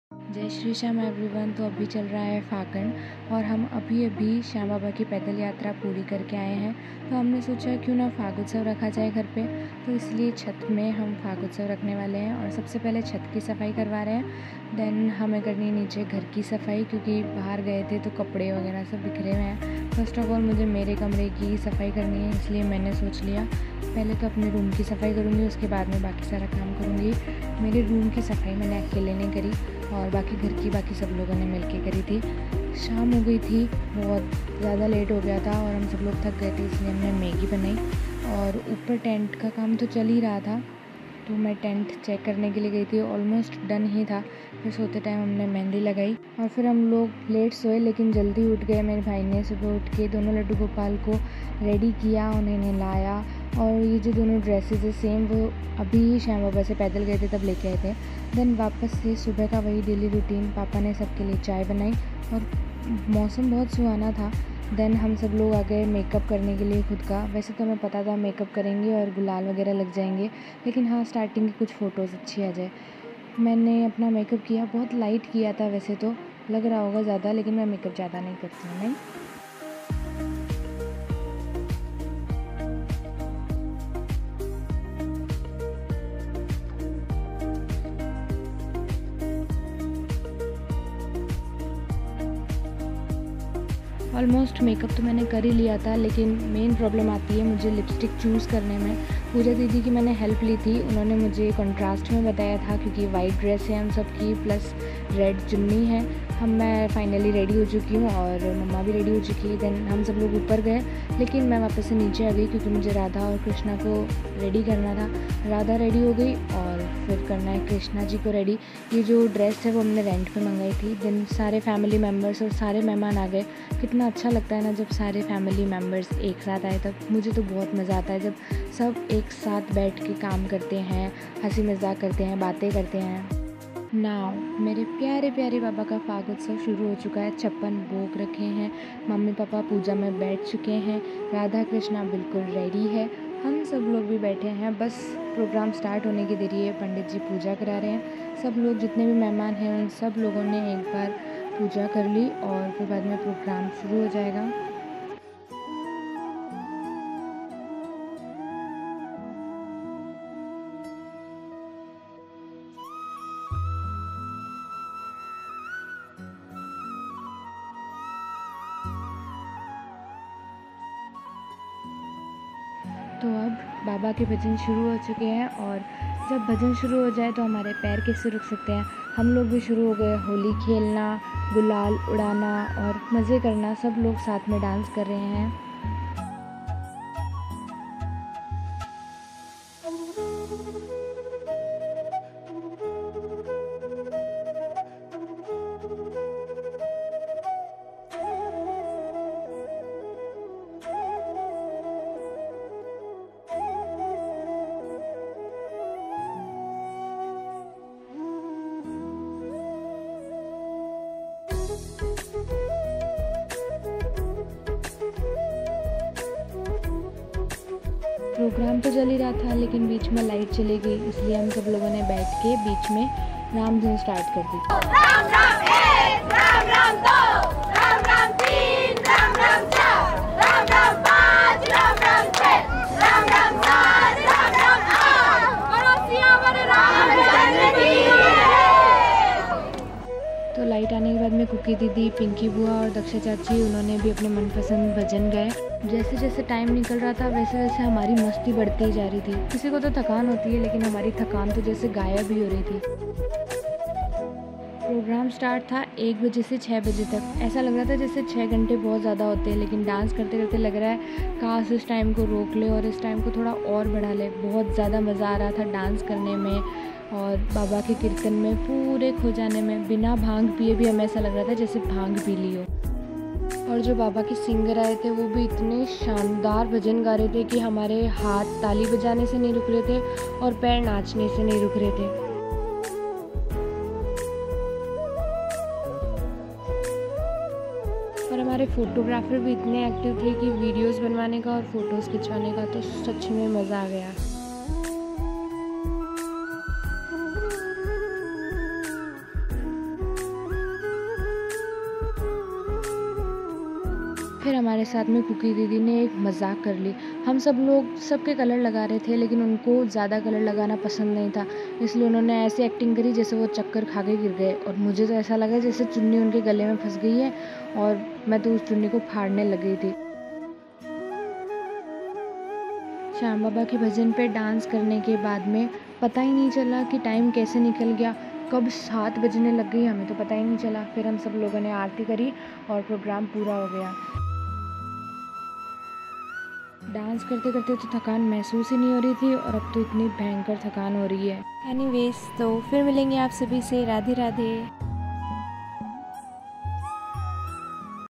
Fagutsav | Fagutsav At Home | Fagun Vlog | Family/Friend Function | Holi Celebration
Fagun Function Management -
→ Photographer » Tarun Photographer
→ Singer » Bhiju Bhai Bhadliya
→ Shyam Baba Decoration » Chandra Prakash Ji Soni
→ Sound » Vikas DJ Sound
→ Tent » Mahavir Tent House
→ Pandit Ji » Mohit Ji Dadhich
→ Water Supply » Shivdhara Child Water